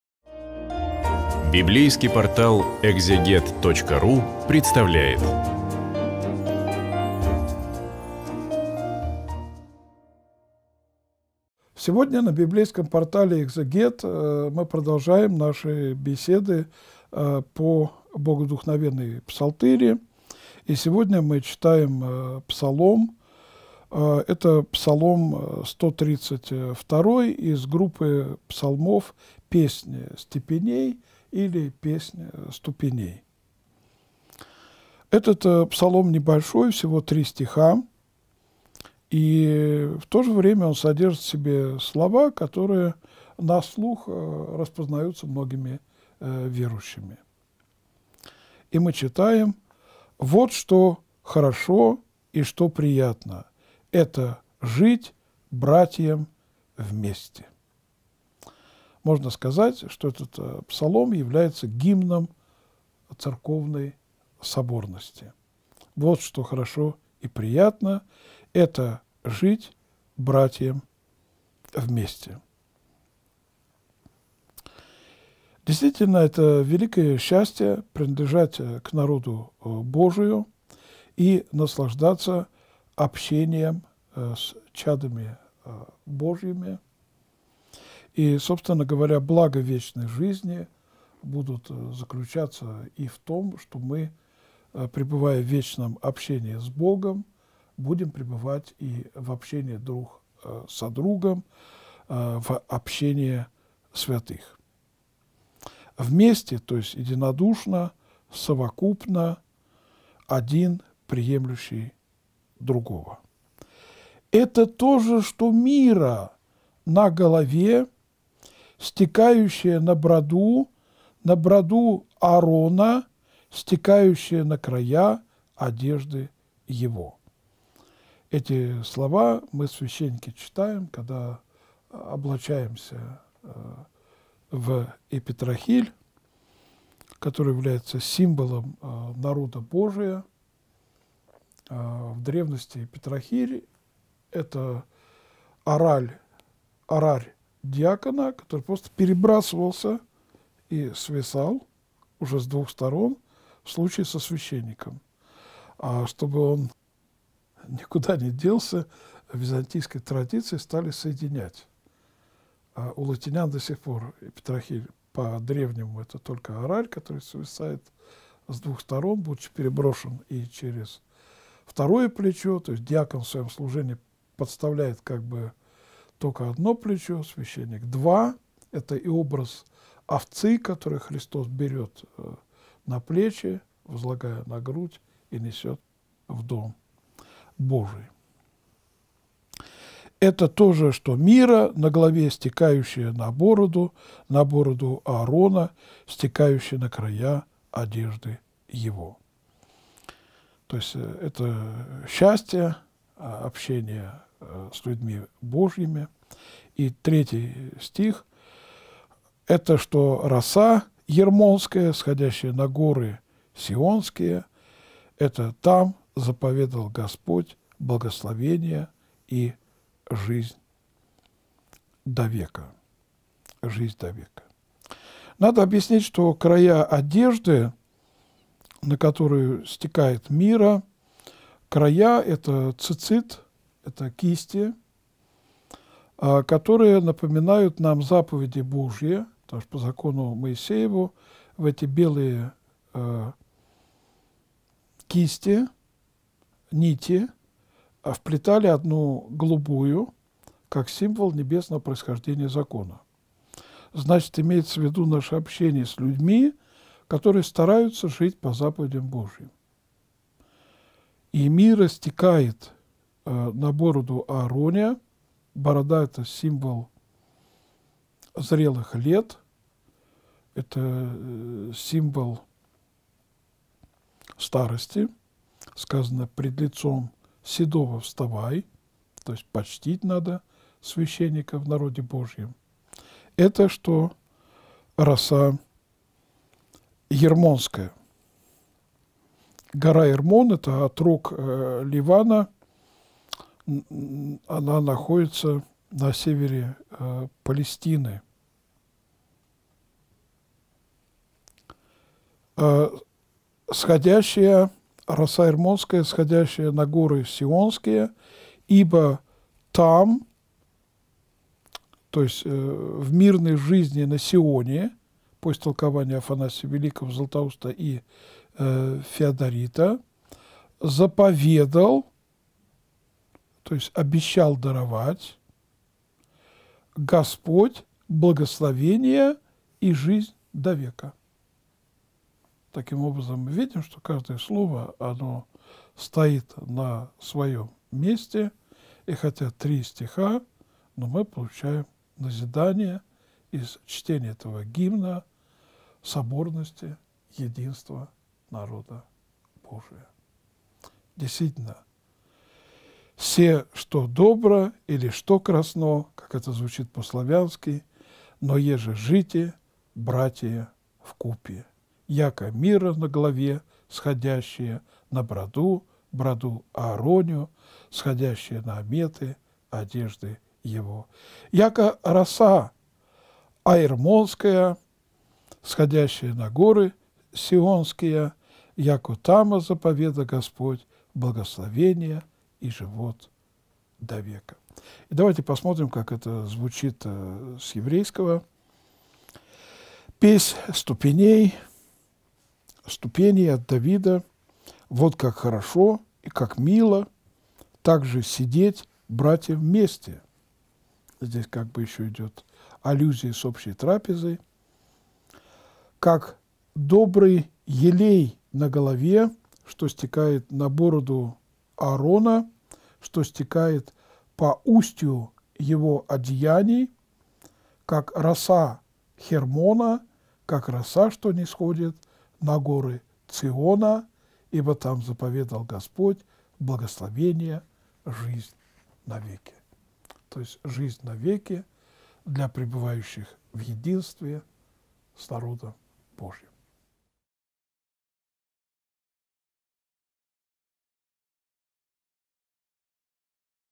Псалтирь. Псалом 132. Протоиерей Олег Стеняев. Библия
Псалом 132 изображает достоинство братского согласия. По какому поводу написан псалом, какие братья в первую очередь имеются в виду? Что значит уподобление братского союза священному елею, стекающему на бороду Ааронову, росе Ермонской, сходящей на горы Сионские? Где заповедал Господь благословение и жизнь на веки?